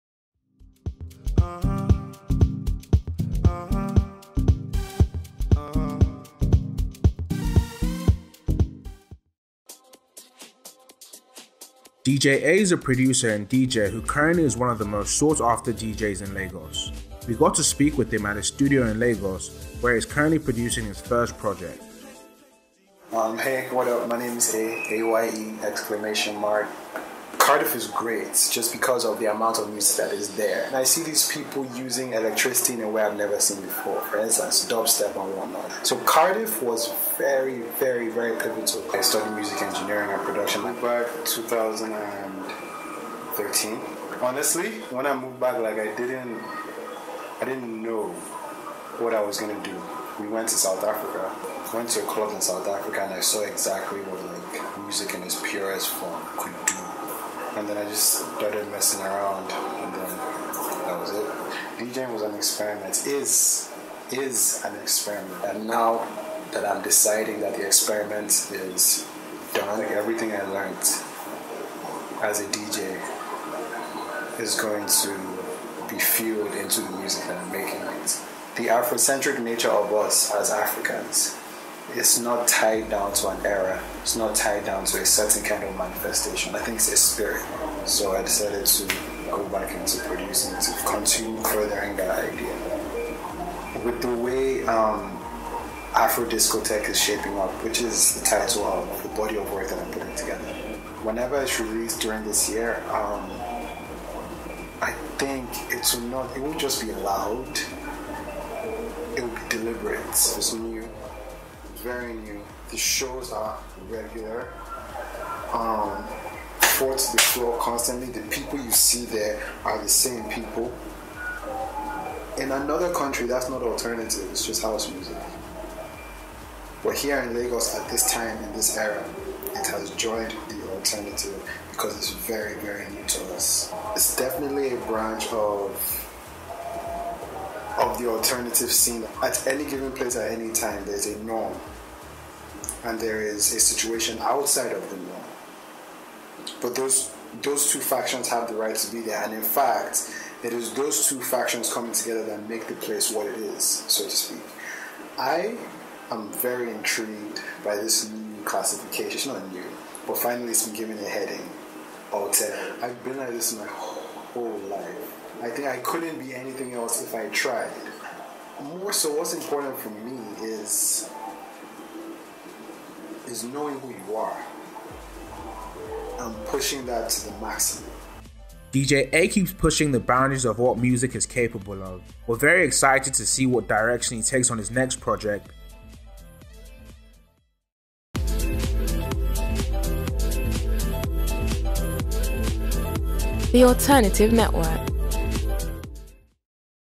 Introducing [S1.EP8]: DJ Aye!
DJ Aye is a producer and DJ who currently is one of the most sought after DJ’s in Lagos. We got to speak with him at his studio in Lagos where he is currently producing his first project.